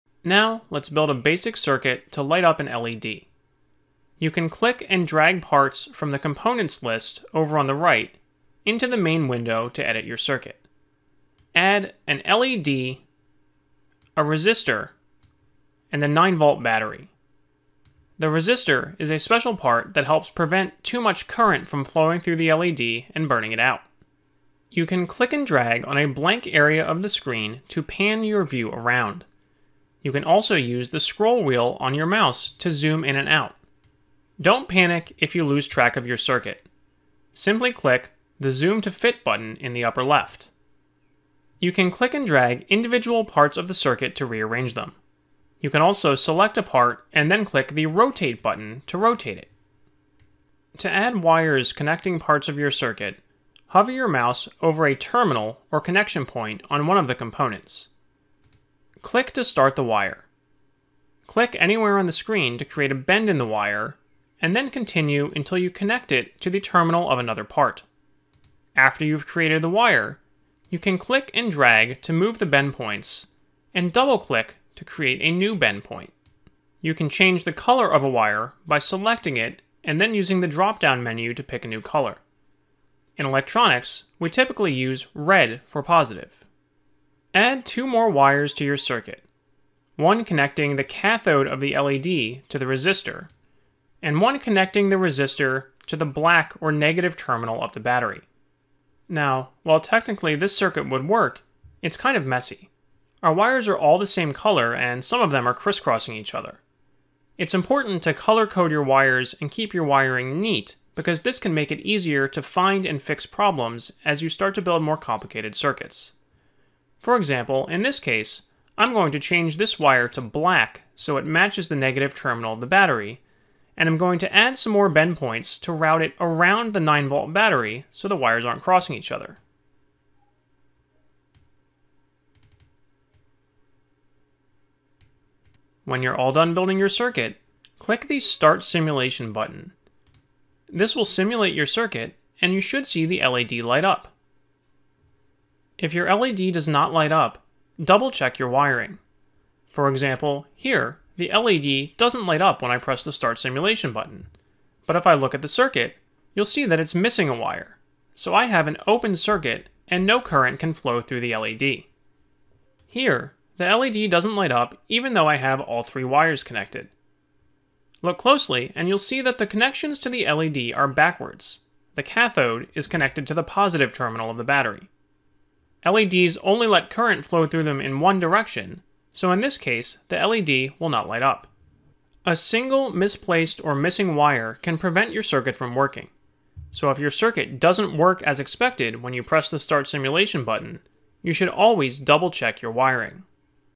LED Light Show #3: Build a Basic LED Circuit | Tinkercad Circuits Lesson Plan
Students will learn how to build a circuit, program an Arduino, and make their own LED light show. This video series is part of a Tinkercad Circuits lesson plan developed as a collaboration between the Cornell University Sibley School of Mechanical and Aerospace Engineering and Autodesk. The full version of the lesson plan, including teacher notes, downloadable Powerpoint slides, and a PDF version is available